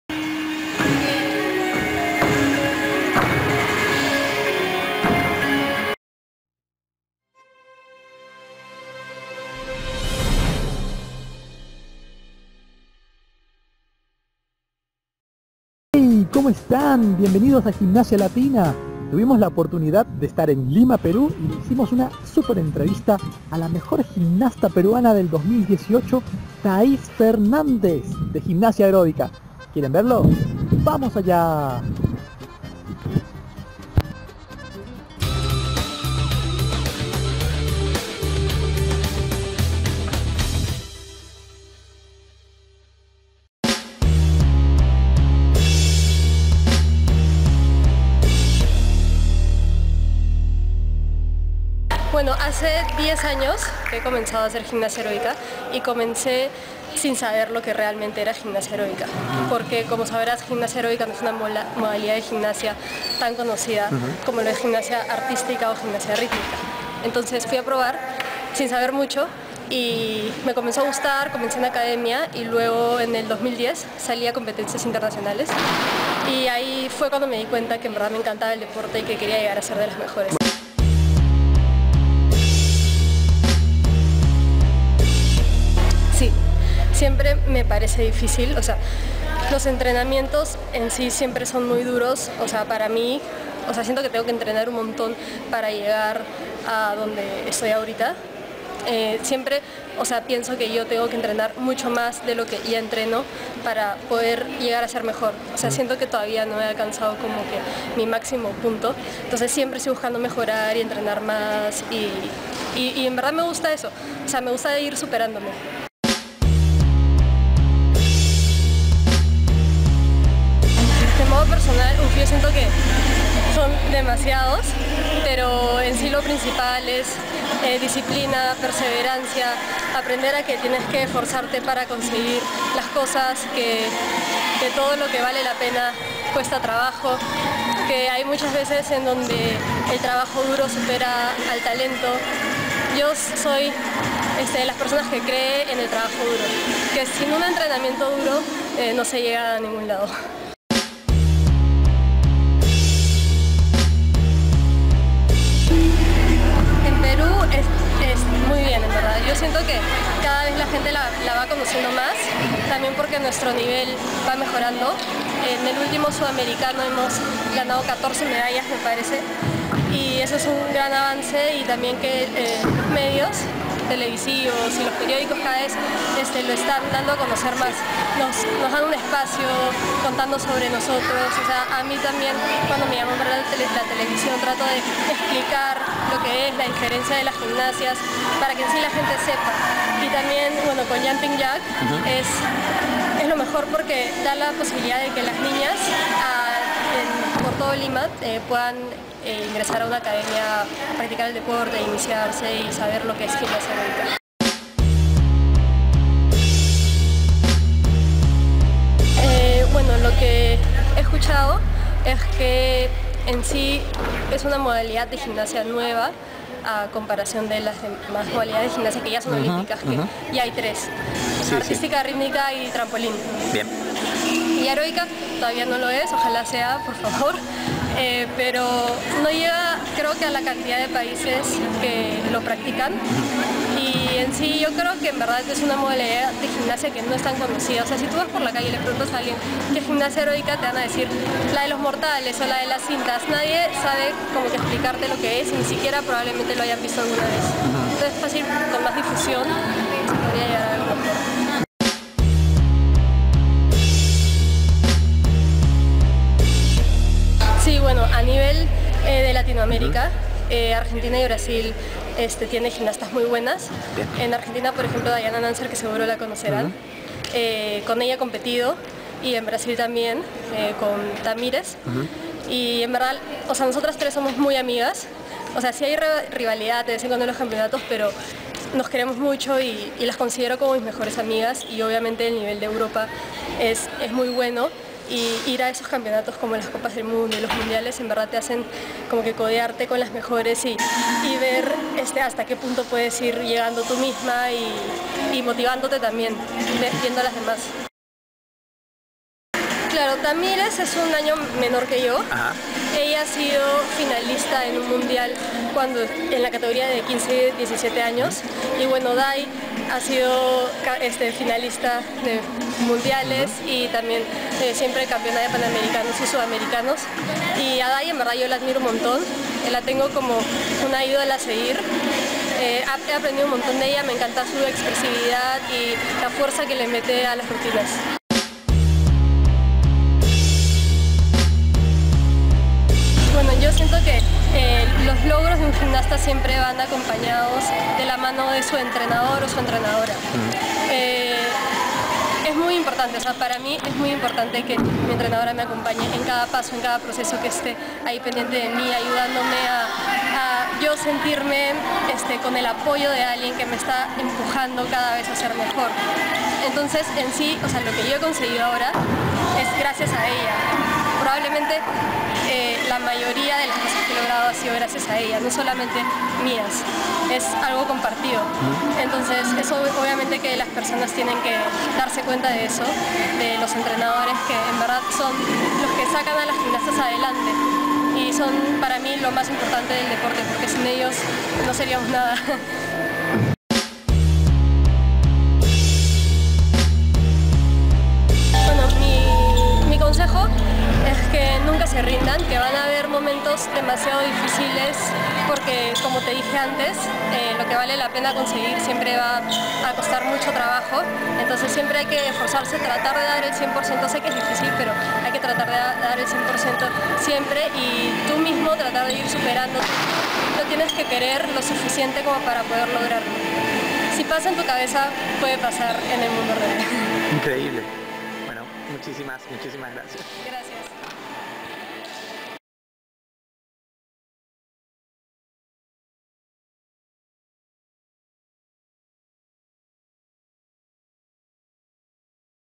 Thais Fernández, la mejor gimnasta de Perú 2018
Tuvimos la oportunidad de estar en Lima, y hacer una entrevista exclusiva con Thais, donde nos habló de sus inicios, sus proyecciones presentes y sus proyecciones futuras.